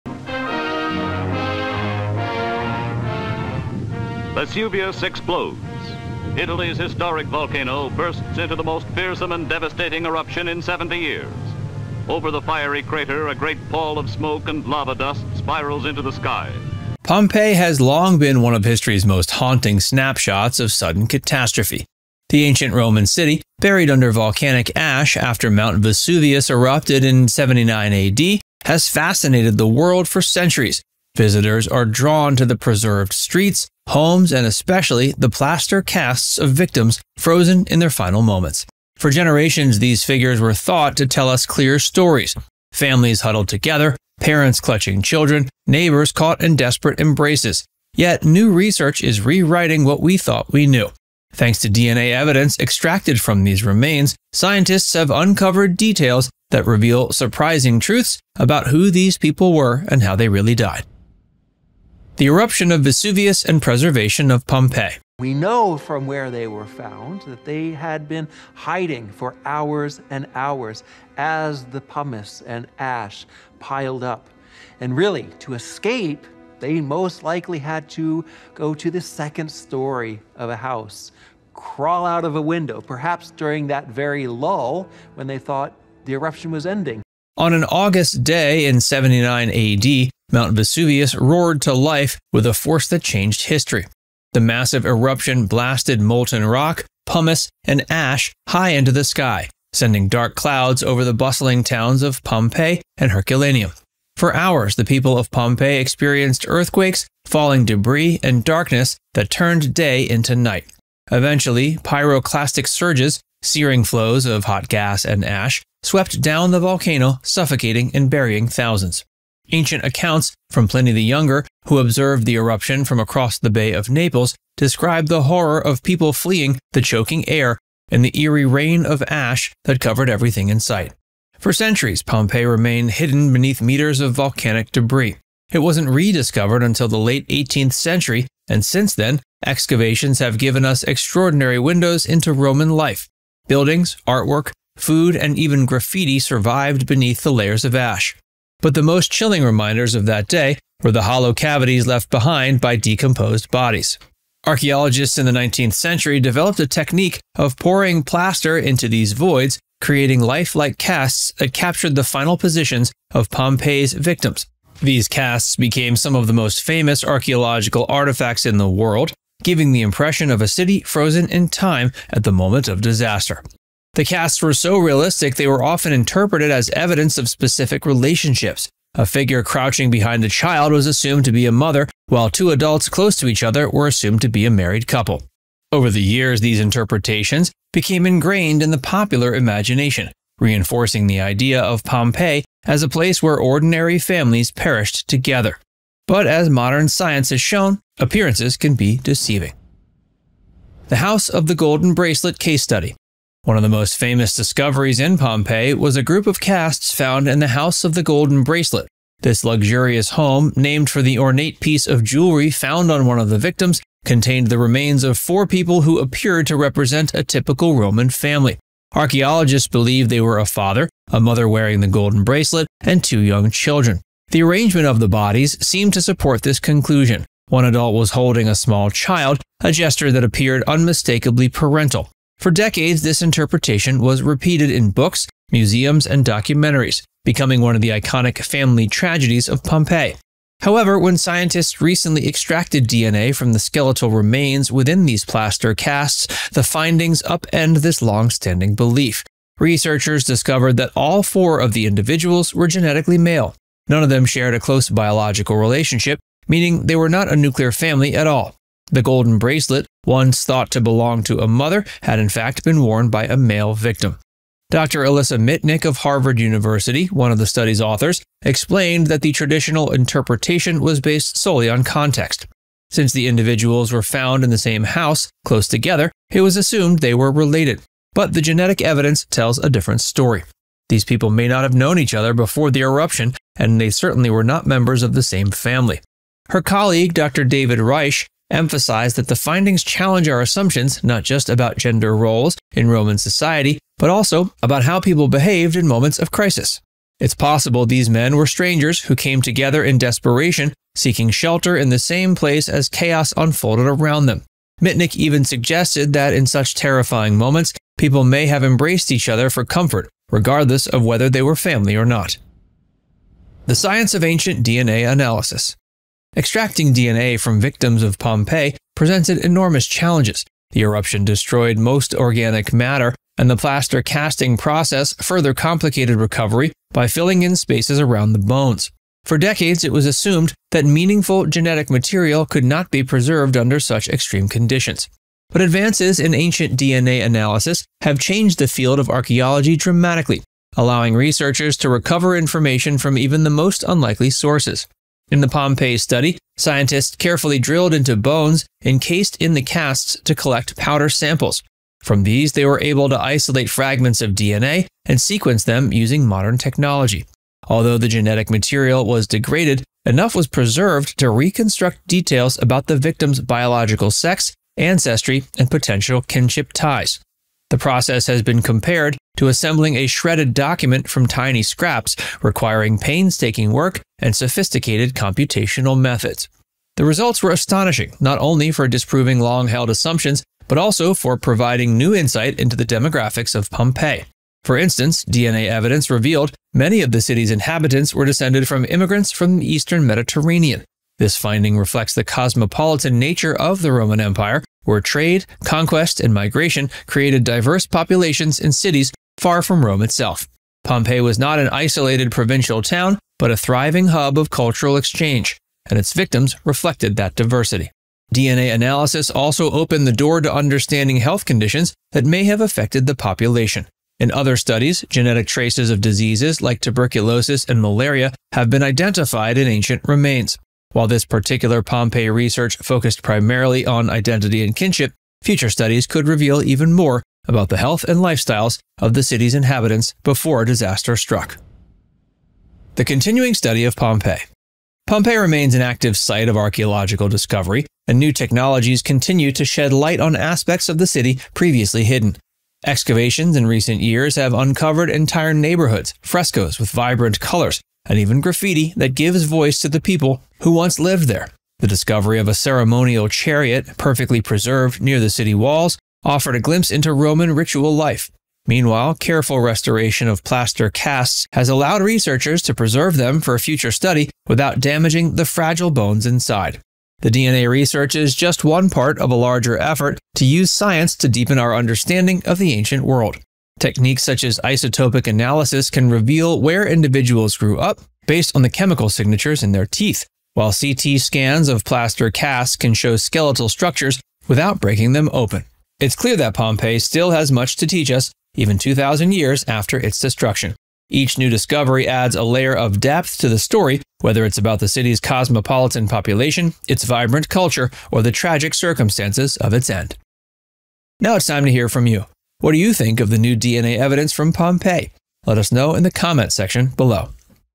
New Pompeii DNA Evidence Reveals How They REALLY Died
Did you know that the plaster casts of Pompeii’s victims, long thought to show families perishing together, may not be what they seem? Did you know that new DNA evidence has revealed surprising truths about who these people really were?

00:00 - Intro
01:02 - The Eruption of Vesuvius and the Preservation of Pompeii
03:20 - The House of the Golden Bracelet Case Study
05:13 - The Science of Ancient DNA Analysis
07:12 - The Continuing Study of Pompeii
08:21 - Outro

When Mount Vesuvius erupted in 79 AD, it unleashed one of the most devastating natural disasters in history. Pompeii, a bustling Roman city, was buried beneath ash and pumice, freezing an entire community in its final moments. For centuries, archaeologists and visitors interpreted the plaster casts of victims as families—parents holding children, couples clinging to each other, neighbors sheltering together. These haunting images became central to our understanding of Pompeii’s tragedy.

But groundbreaking research using ancient DNA has changed that story. By analyzing genetic material from victims, scientists discovered that one famous group thought to be a mother, father, and two children were in fact unrelated men. This shocking finding challenges assumptions about gender, family, and survival in moments of disaster. It also reveals more about Pompeii’s diverse population, with many residents descended from immigrants across the Mediterranean, reflecting the cosmopolitan nature of the Roman Empire.

This video explores the eruption itself, the history of excavations, the case of the House of the Golden Bracelet, and the science of ancient DNA that is rewriting history. We’ll look at how narratives shaped by visual impressions gave way to the hard evidence of genetics, and how Pompeii continues to reveal secrets more than two thousand years later. The victims of Vesuvius are no longer just symbols frozen in plaster but individuals whose lives and deaths were far more complex than previously imagined.